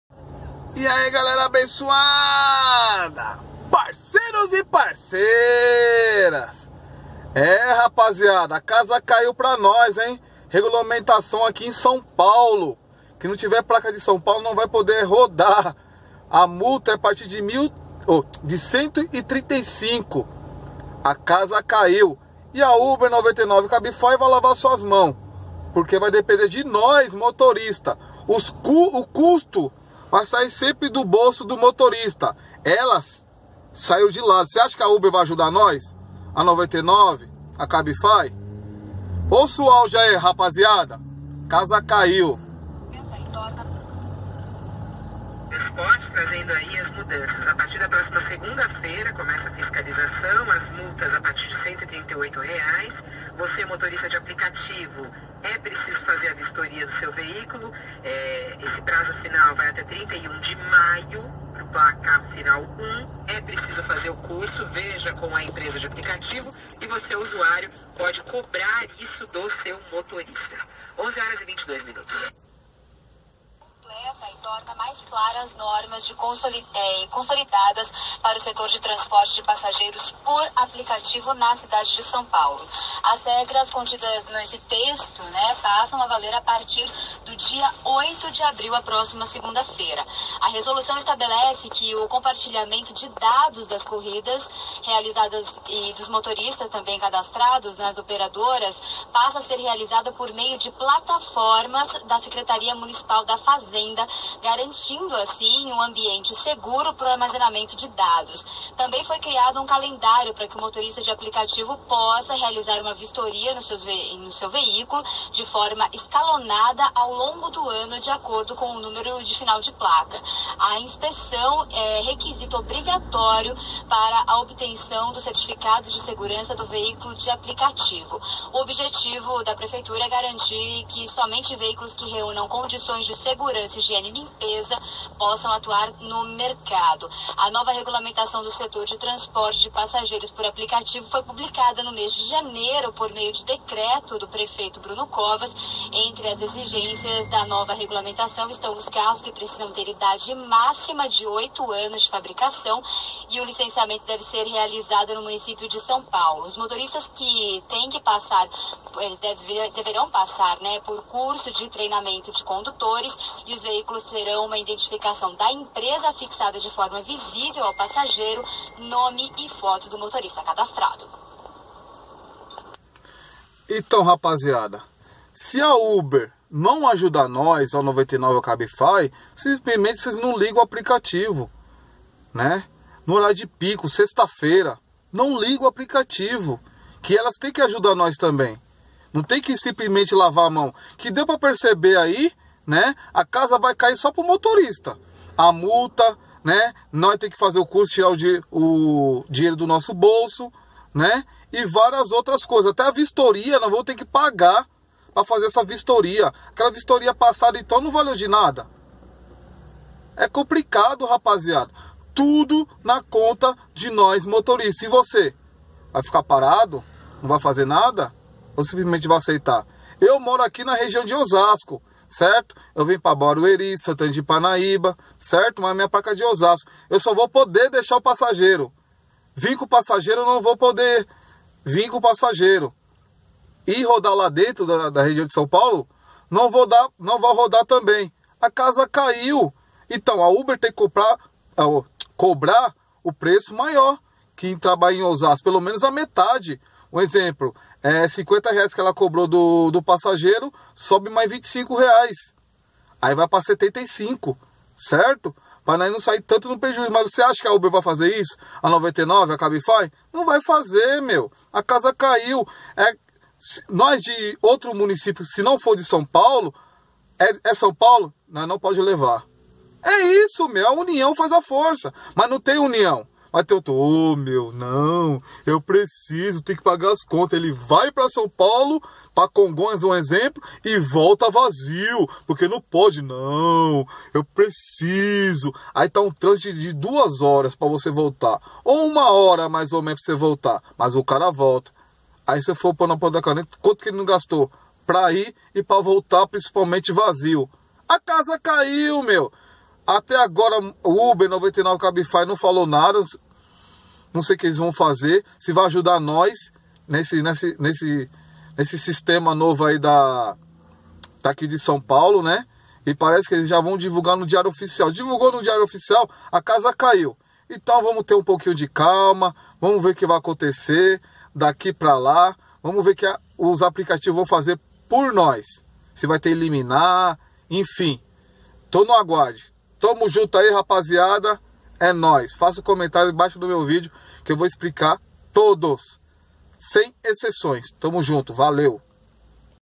🗣️ novas regras para os aplicativos em São Paulo 2019 Uber #99
Esse novo procedimento infelizmente só prejudica nós motorista vamos ver o que os aplicativos vão fazer